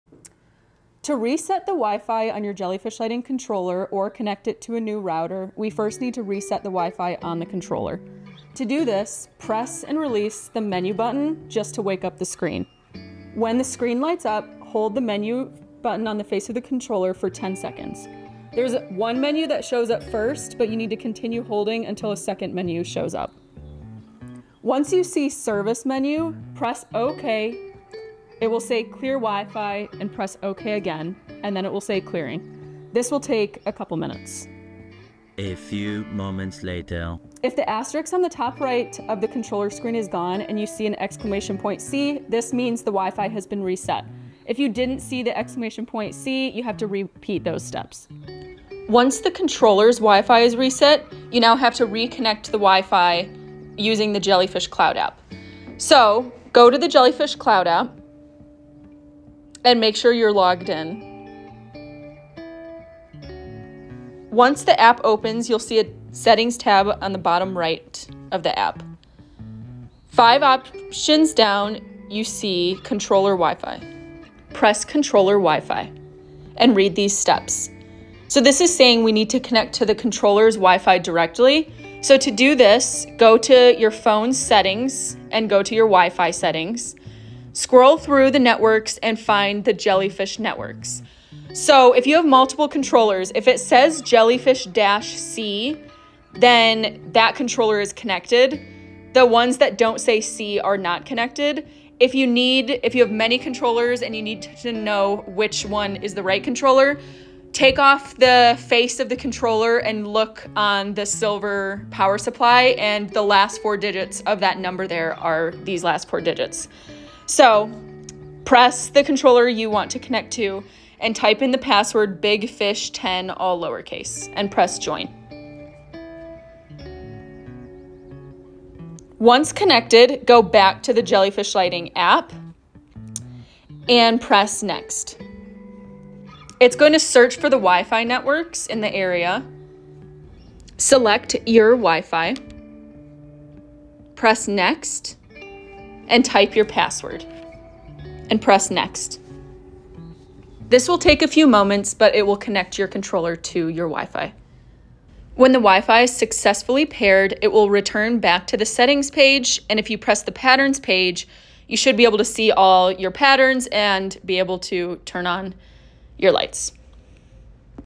Reset WiFi or Connect JellyFish Controller to a Different Router
To reset the Wi-Fi on the controller:

Press and release “MENU” on the face of the controller to wake up the screen.

When the screen lights up, hold down “MENU” on the face of the controller for 10 seconds for controllers that are the version 030113+, or 3 seconds for controllers that firmware version start with 02.

The screen will jump to a set up menu, then a service menu. Remember to keep holding the menu button when you see ‘Set Up’ until you see ‘Service Menu’.

In the service menu, the first option will be ‘Reset WiFi’

Press OK and it will say ‘Clear WiFi?’

Press ok and it will say ‘Resetting WiFi’.

This will take a minute.

If the asterisk on the top right of the controller screen is gone and you see !C, then it has reset, if it is still there you will need to repeat these steps.

Reconnect your controller to WiFi using the JellyFish Cloud App

You will need to open the JellyFish Cloud app and log in

Once the app opens you will see a settings tab on the bottom right, click that tab.

5 options down you will see Controller WiFi, click that option.

It will bring you to a page with 3 steps, read through the steps while on that page and when you get to step 3 you will need to leave the JF cloud app open and jump to the devices WiFi settings.

Connect your phone to the JellyFish-xxxx Wi-Fi, Password bigfish10.

Press "Next”.

Press “Search for networks” and click on your network.

Keep in mind the controller needs a 2.4 GHZ wifi to connect to and will not properly connect to 5.0 GHZ WiFi.

After selecting the appropriate network you will need to press NEXT in the top right corner to progress to the next page where you will type in the password for the selected WIFI network that you just chose.

Reconnect your controller to WiFi using the JellyFish Designer App

Press the “Setup” tab in the Designer app.

Press “Wi-Fi Setup”.

Type in your Network Password and press connect.

It will say “Configuring Wi-Fi…” and will take about 5 minutes.

An * means the controller is connected and working.

*9 is a perfect connection, *8 is slightly less and so on, a *6 or higher is good.

Anything under *6 will need to have a WiFi extender installed to create a more stable connection for the controller and router to communicate through.